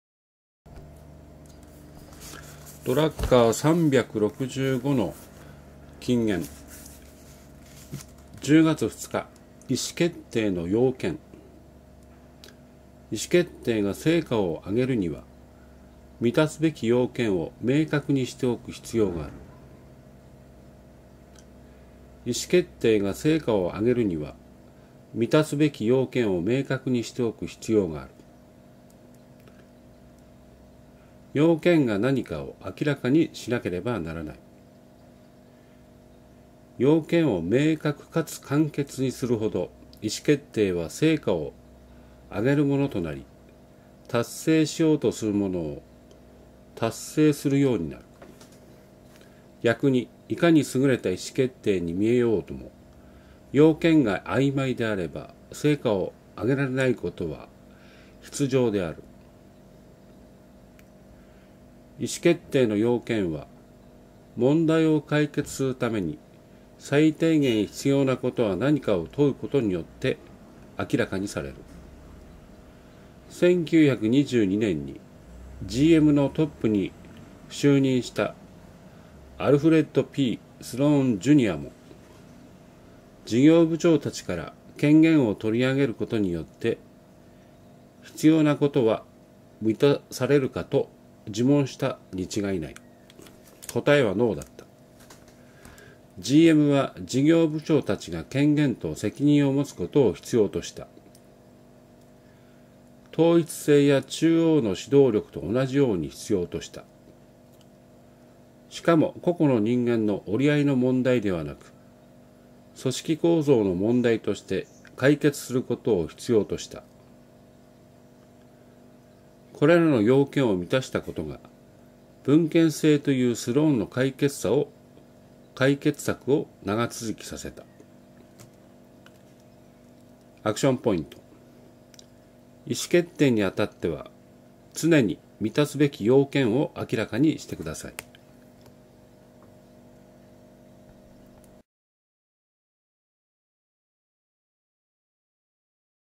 意思決定にあたっては、 常に満たすべき要件を明らかにしてください。
S.L.1052
意思決定の要件　10月２日朗読ドラッカー　The Daily Drucker
意思決定が成果をあげるには
満たすべき要件を明確にしておく必要がある。
要件が何かを明らかにしなければならない。
要件を明確かつ簡潔にするほど、意思決定は成果を
あげるものとなり、達成しようとするものを達成するようになる。
意思決定の要件は、問題を解決するために最低限必要な
ことは何かを問うことによって明らかにされる。
意思決定にあたっては、
常に満たすべき要件を明らかにしてください。
背景　台風一過三鷹市野川の陽光
経営者の条件